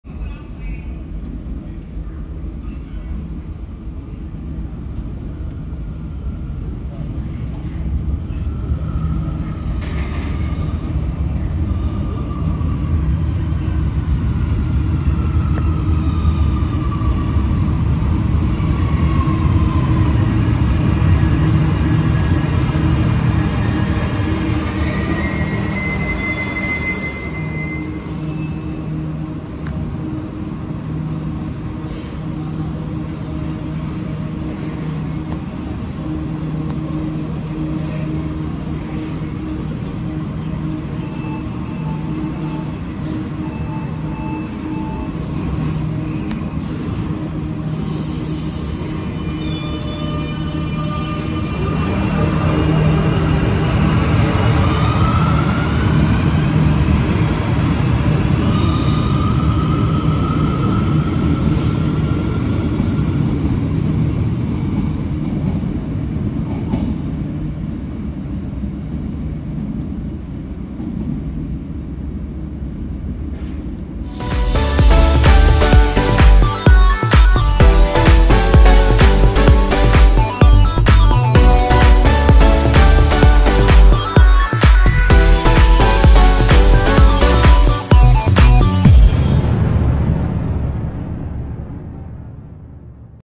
Subte de Buenos Aires | Alstom RC322 en Bulnes
Grabación del Tren Alstom Serie 300 RC322 entrando y saliendo de Bulnes en dirección Congreso de Tucuman.

Bulnes.
Linea D, Subte de Buenos Aires.